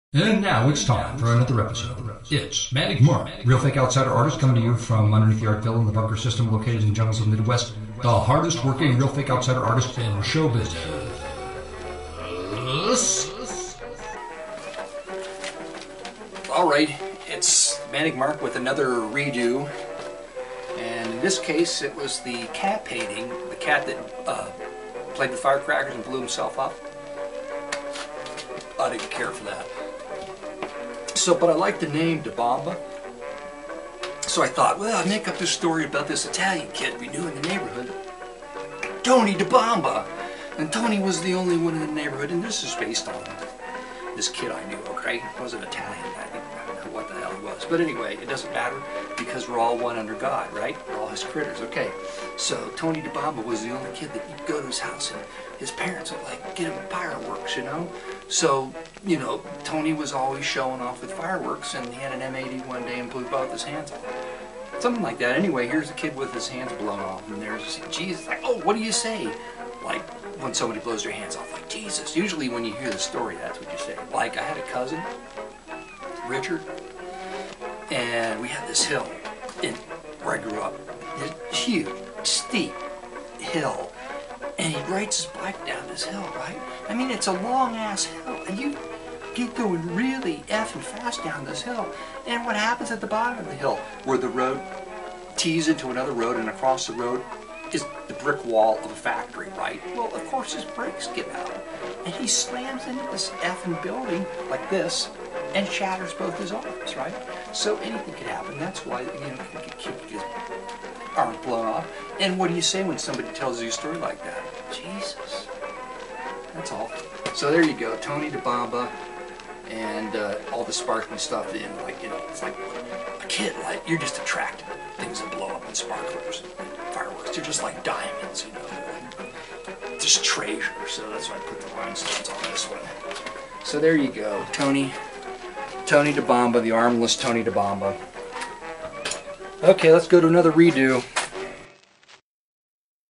Tony Debomba REDEW Playing With FireWorks Blowing You Hands Off JESUS!
OK... now I have this new character... Tony Debomba... Tony will be a kid from the neighbor back in the 60s with those little clamps for arms who folks STILL supply him with fireworks even after the tragic accident. Tony is the coolest kid in school!